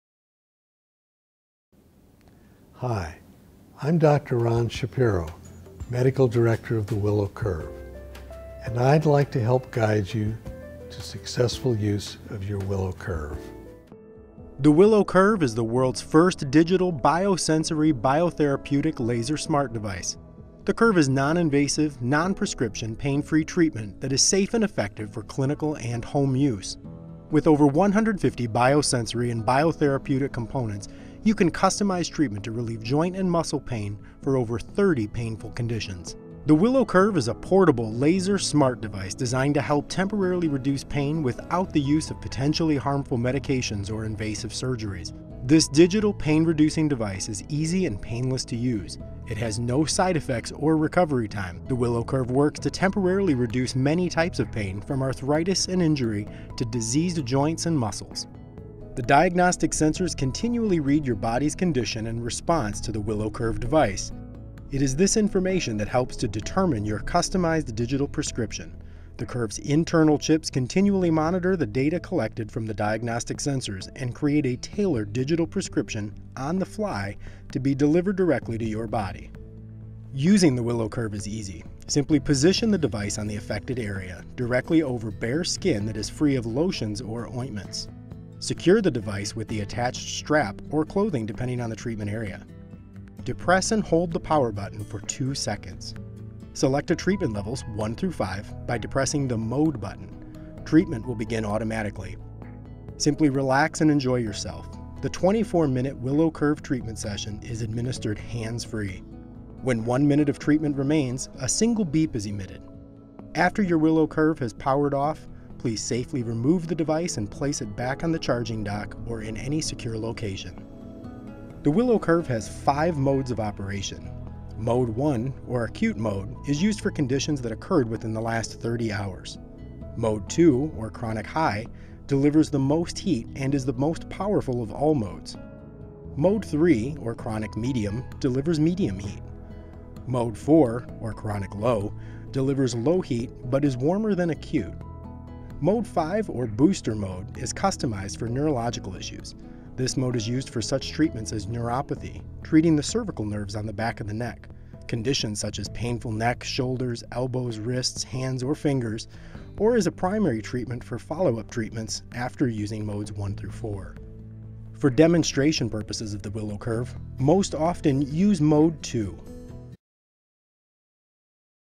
The Willow Curve™ Home Laser Treatment Devices
Video transcript: Hi I’m Doctor Ron Shapiro, Medical Director of the Willow Curve and I would like to help guide you to successful use of your Willow Curve.

The Willow Curve is the first digital bio-sensory bio-therapeutic laser smart device. The Curve is non-invasive, non-prescription pain free treatment that is safe and effective for clinical and home use. With over 150 bio-sensory bio-therapeutic components you can customize treatment to relieve joint and muscle pain for over 30 painful conditions. The Willow Curve is a portable laser smart device designed to temporally reduce pain without the use of potentially harmful medications or invasive surgeries.

This digital pain reducing device is easy and painless to use, it has no side effects or recovery time. The Willow Curve works to temporarily reduce many types of pain from arthritis and injury to diseased joints and muscles.

The diagnostic sensors continually read your bodies’ condition and response to the Willow Curve device. It is this information that helps to determine your customized digital prescription. The Curve’s internal chips continually monitor the data collected from the diagnostic sensors and create a tailored digital prescription on the fly to be delivered directly to your body.

Using the Willow Curve is easy. Simply position the device on the affected area, directly over bare skin that is free of lotions or ointments. Secure the device with the attached strap or clothing depending on the treatment area. Depress and hold the power button for two seconds. Select a treatment level from 1-5 by depressing the mode button. Treatment will begin automatically. Simply relax and enjoy yourself. The 24 minute Willow Curve treatment session is administered hands-free. When one minute of treatment remains a single beep is admitted. After your Willow Curve has been powered off, please safely remove the device and place it back on the charging dock or any secure location.

The Willow Curve has five modes of operation. Mode 1 or acute mode is used for conditions that have occurred within the last 30 hours. Mode 2 or chronic high delivers the most heat and is the most powerful of all modes. Mode 3 or chronic medium delivers medium heat. Mode 4 or chronic low delivers low heat but is warmer than acute. Mode 5 or booster mode is customized for neurological issues. This mode is used for such treatments as neuropathy, treating the cervical nerves on the back of the neck, conditions such as painful neck, shoulders, elbows, wrists, hands, or fingers or as a primary treatment for follow up treatments after using modes 1-4.

For demonstration purposes of the Willow Curve most often use mode 2.